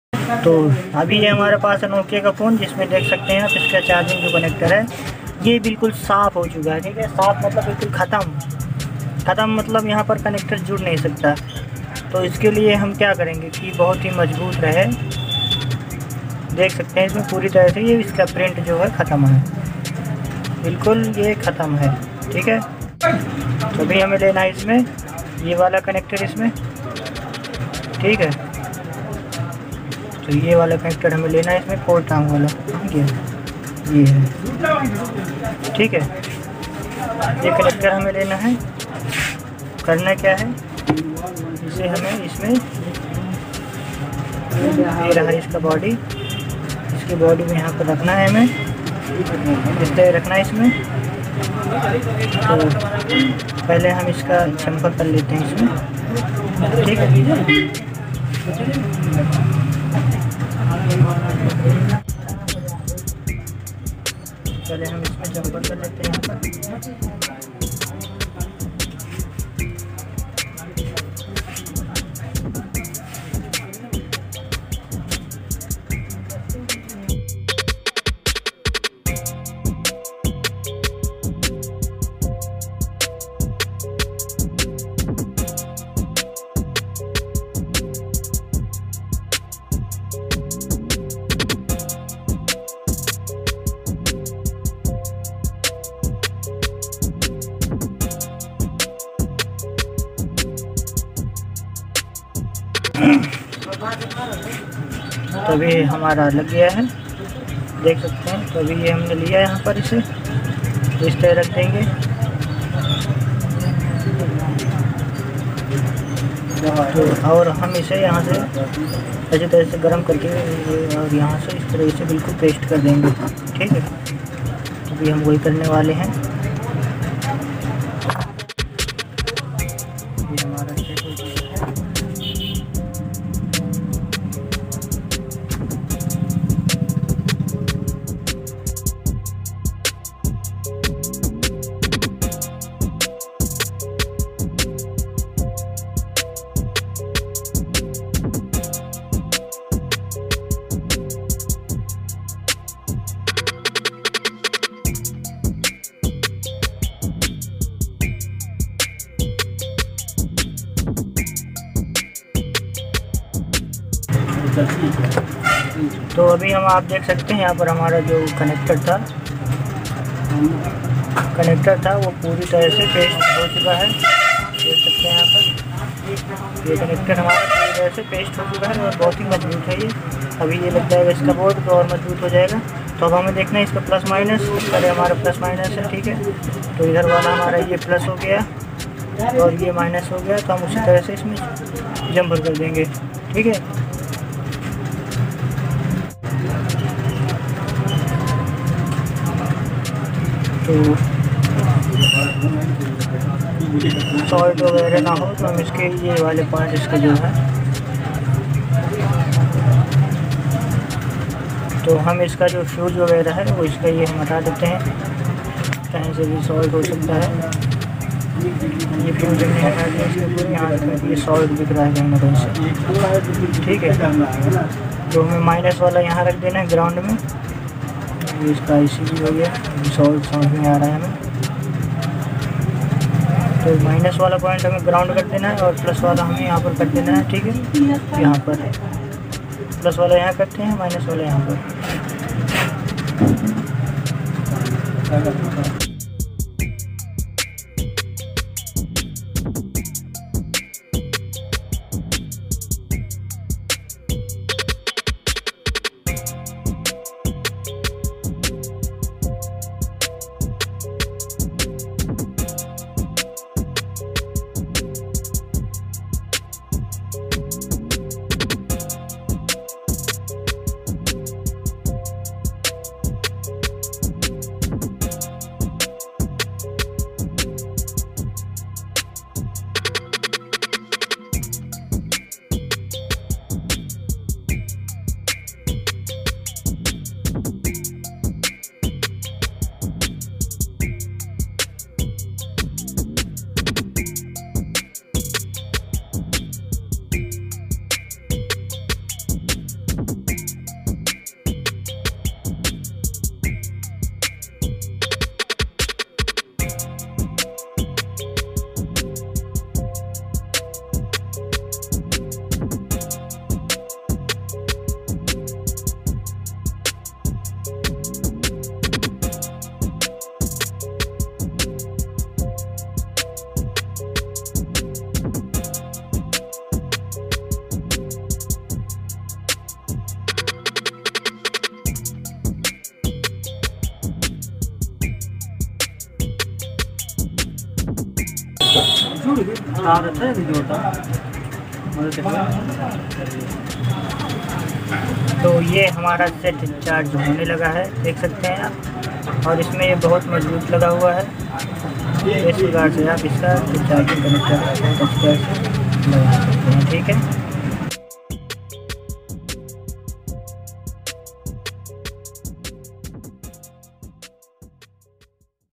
NOKIA 105 NEW Charging Print Damage, How To sold charging connecter New Trick 2022.
NOKIA 105 NEW Charging Print Damage, How To sold charging connecter